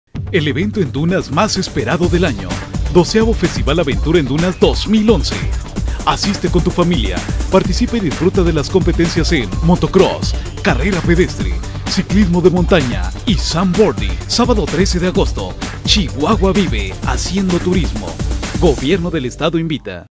AVENTURA EN DUNAS 2011
VIVE LA EMOCION Y LA AVENTURA EN LAS DUNAS DE SAMALAYUCA
EL FESTIVAL DEPORTIVO MAS IMPORTANTE EN EL DESIERTO DE MEXICO